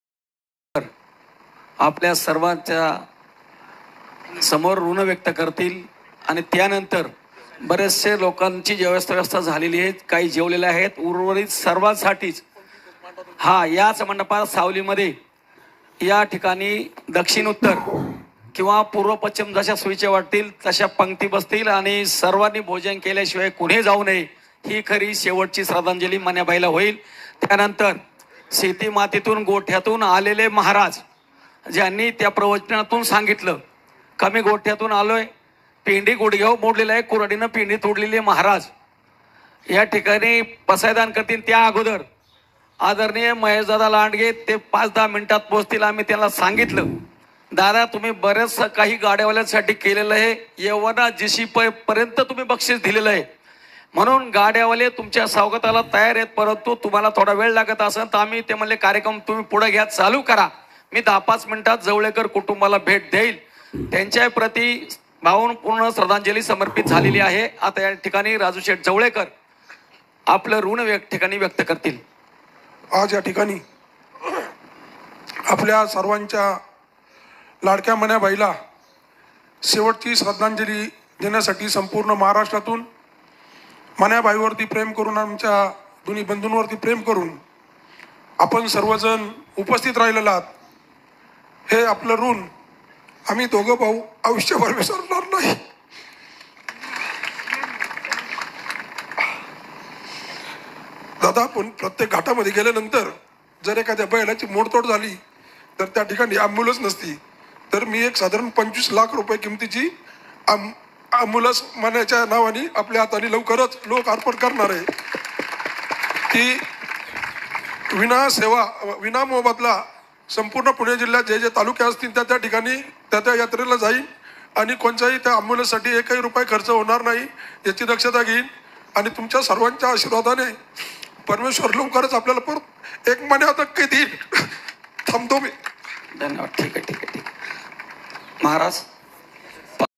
मन्याभाई च्या नावाने राजु शेठ जवळेकर हे एक ambulance 🚑 भेट म्हणून देणार. राजुशेठ झाले भावनिक
vafgav